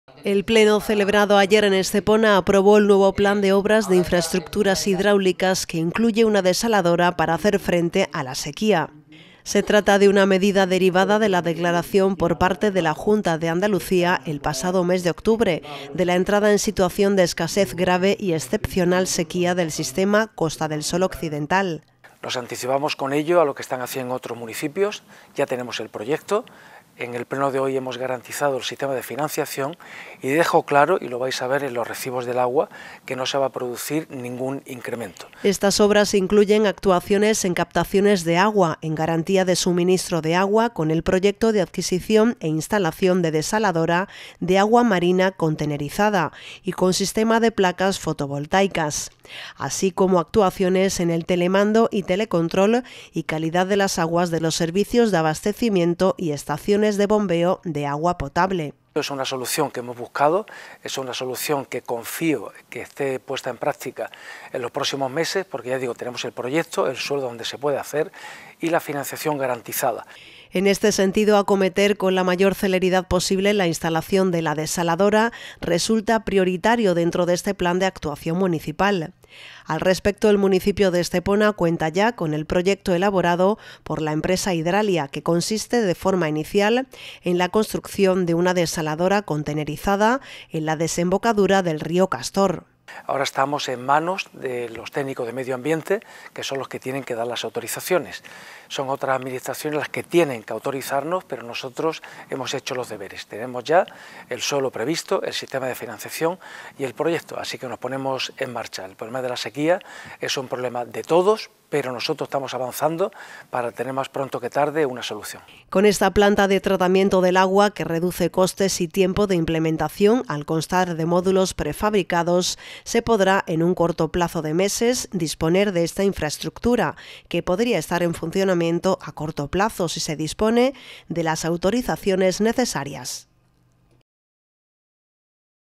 El Pleno aprueba el nuevo Plan de Obras de Infraestructuras Hidráulicas
El Pleno del Ayuntamiento de Estepona aprobó ayer el nuevo Plan de Obras de Infraestructuras Hidráulicas, que incluye una desaladora para hacer frente a la sequía